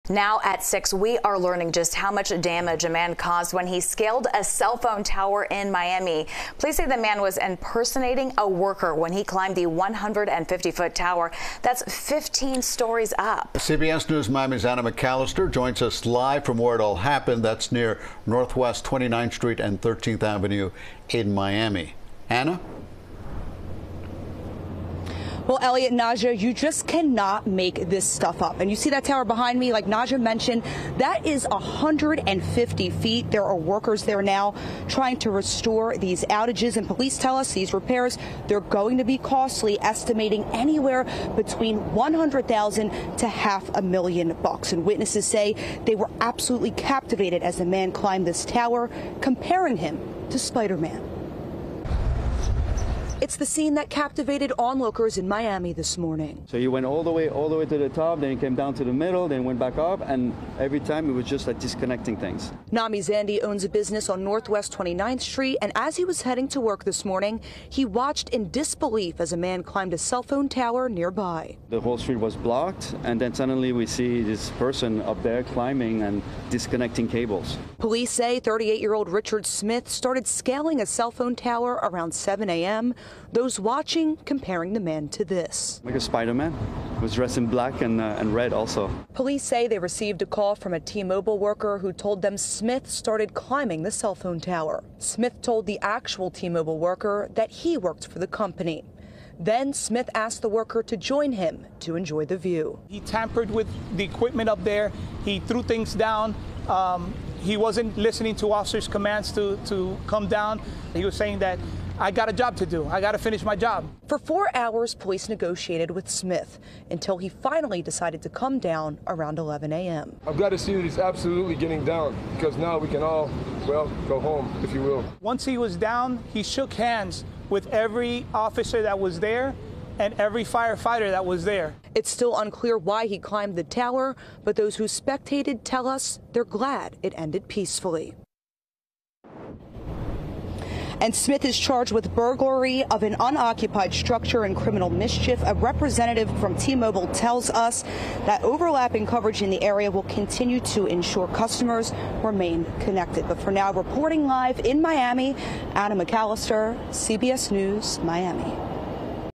Man posing as T-Moblie worker climbed Miami cell tower, refused to come down
CBS News Miami's Anna McAllister reports from the scene after a man climbed a Miami cell tower and refused to come down.